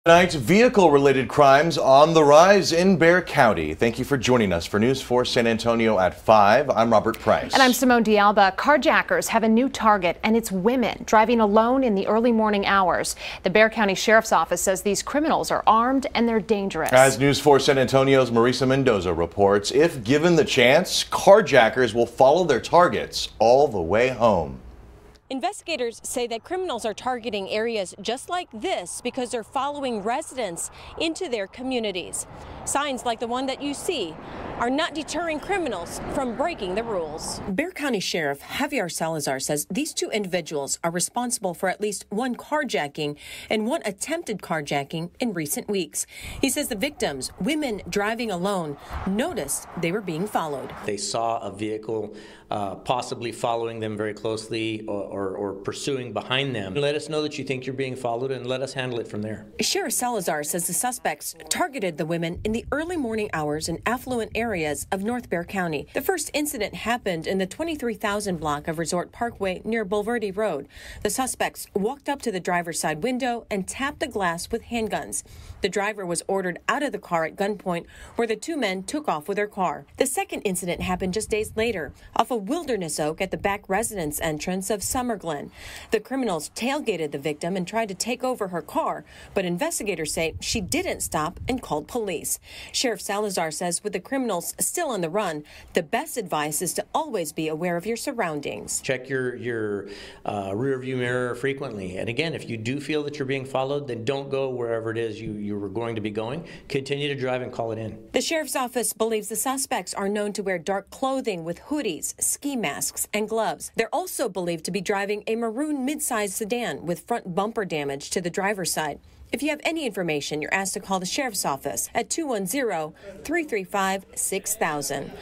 Beware of carjackers who are targeting women driving alone, Sheriff Salazar says
Carjackers have a new target, and it is women driving alone in the early morning hours. The Bexar County Sheriff’s Office says these criminals are armed and dangerous. Bexar County Sheriff Javier Salazar says if given the chance, carjackers will follow their targets all the way home.